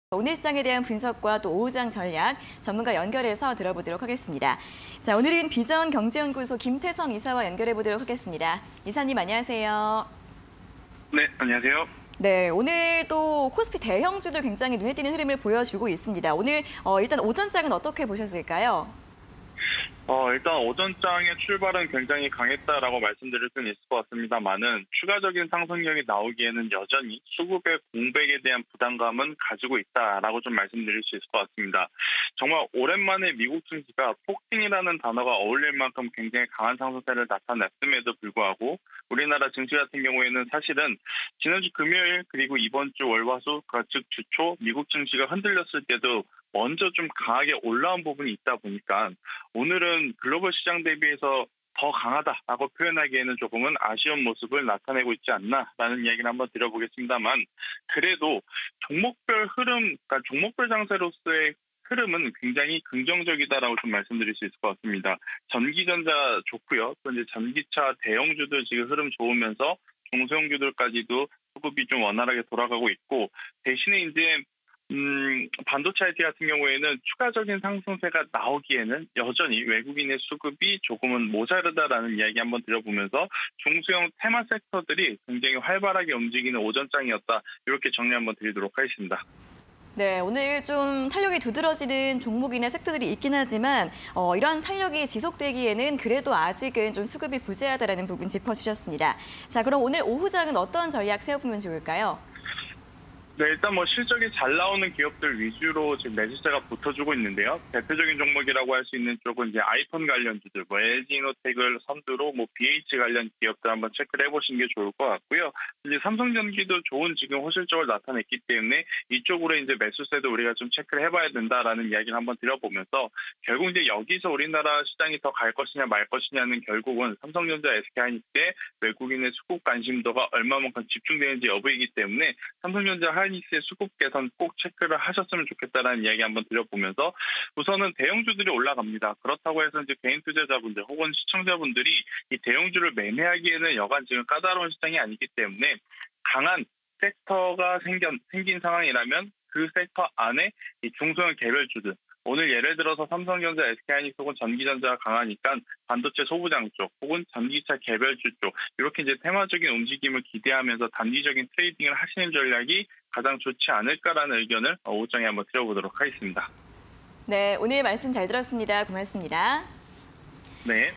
삼성전자·SK하이닉스 수급 개선 체크_시장 TALK (20220728)
- 실적에 따른 종목장세 심화
- 전기전자·전기차 등 관련주 강세
- 수급 부재는 여전

- 삼성전자·SK하이닉스 수급 개선 체크
- 반도체 소부장 및 전기차 관련 개별주 위주 단기 트레이딩 집중